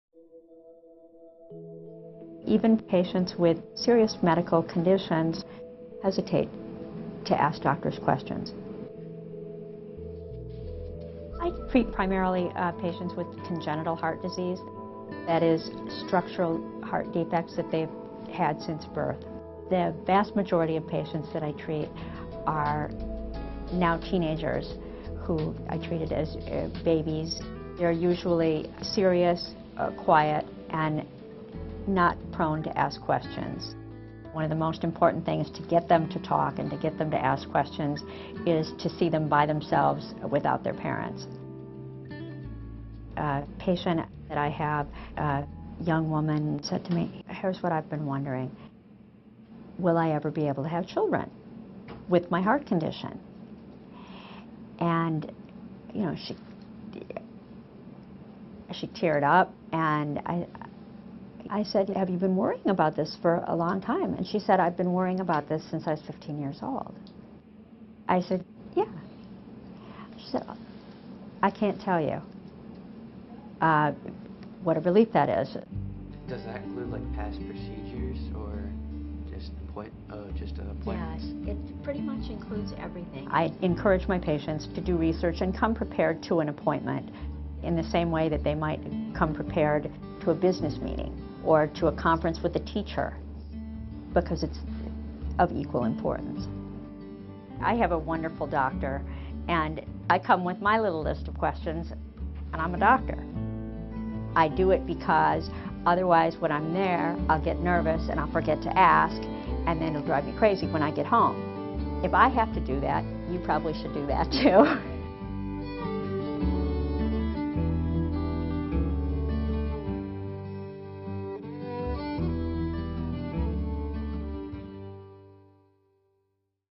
Congenital Heart Disease Management | Children's National Health System
Cardiologists at Children's National developed Follow My Heart to help patients take control of their congenital heart disease. It was featured on AHRQ's Questions are the Answer campaign.